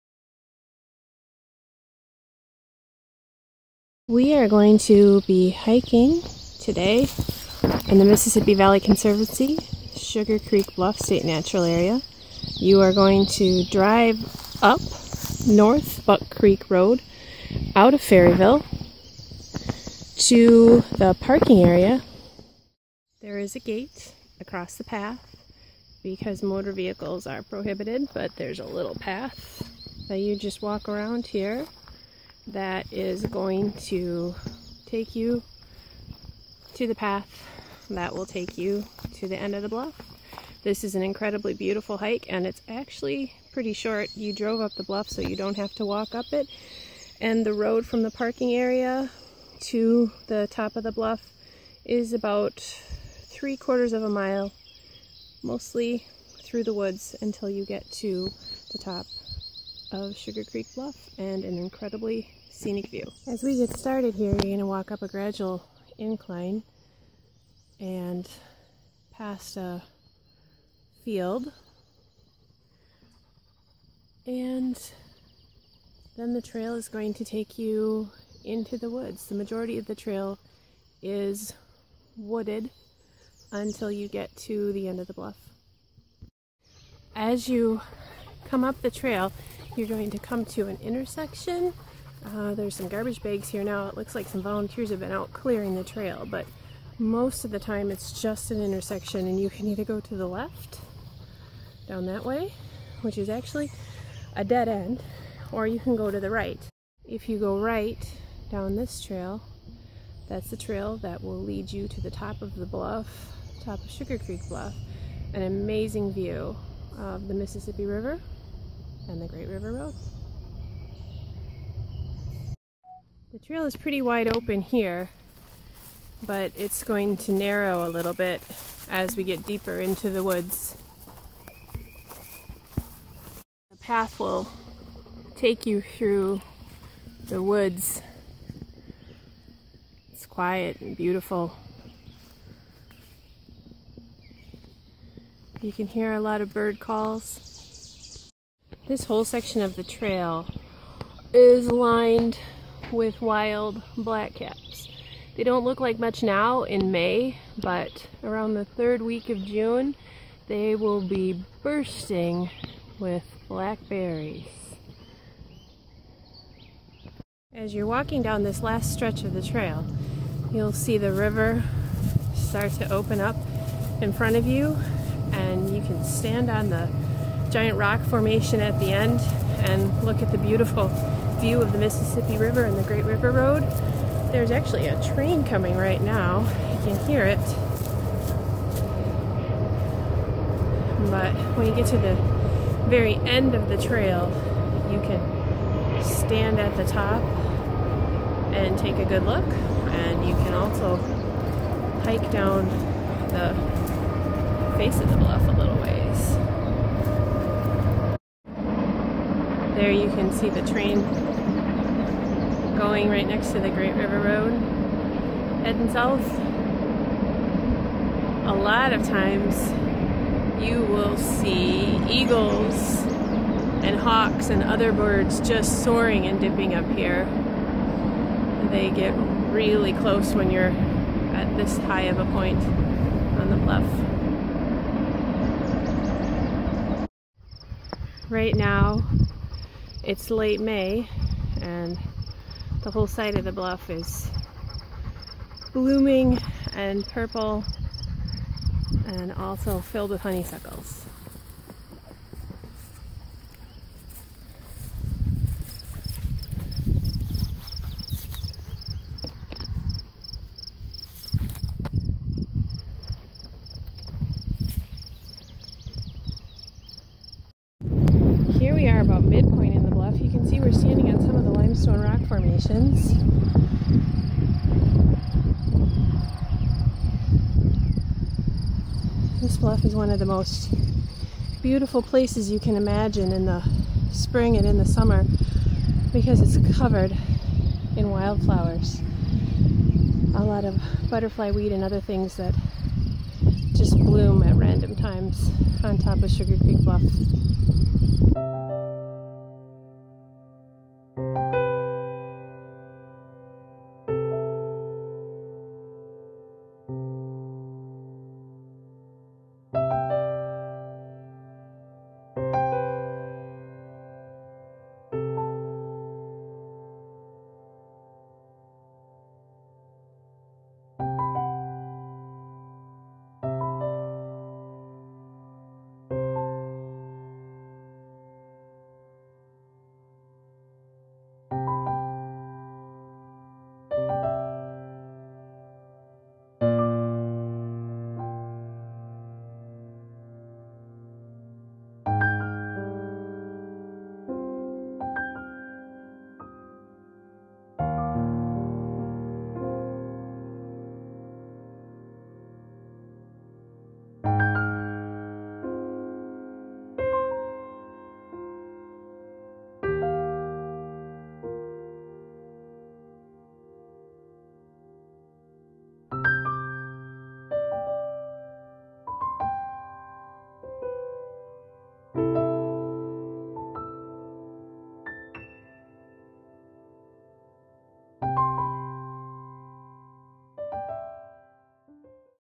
Hiking Sugar Creek Bluff
Video of the hiking trail that leads from the parking lot on North Buck Creek Road to the outlook at the end of Sugar Creek Bluff. This land is part of the Mississippi Valley Conservancy in Ferryville, Wisconsin and free and open to the public. For more information or a map of the area,

For more information about hiking and other outdoor activities in the area,